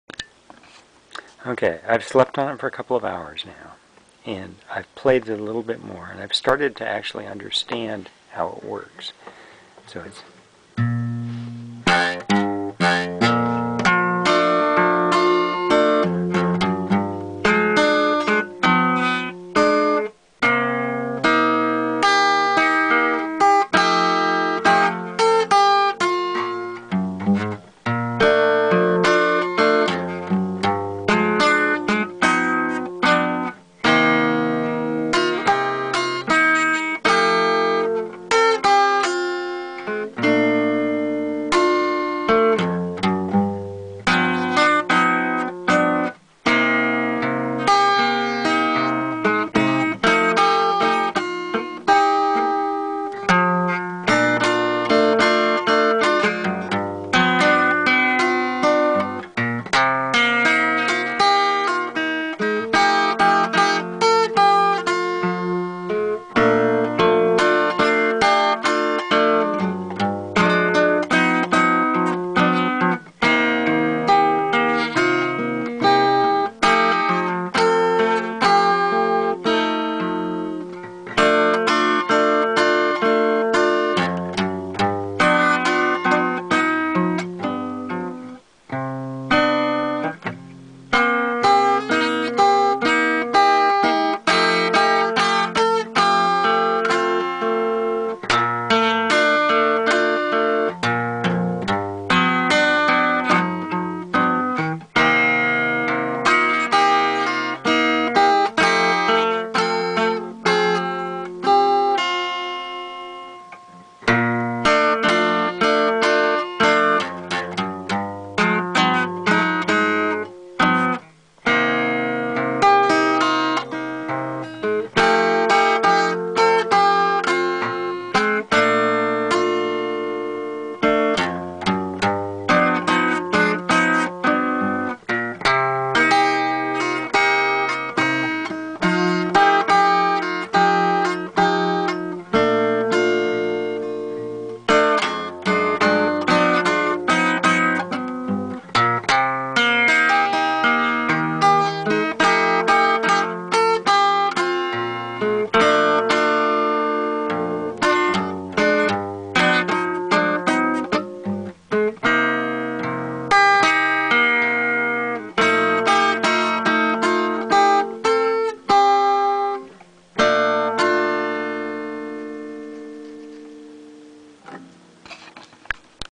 continual evolution of the upside-down chord stuff
this sequence represents a deliberate attempt to develop new wiring. it happens fast, too, but my guess is that pretty much anyone could pick up a guitar and learn to finger and use these shapes abut as fast as I can - cuz nearly everything I know how to play gets in the way of playing them smoothly. But they keep showing me new stuff. Like this example of what just one of those changes can do, if it's used in a different way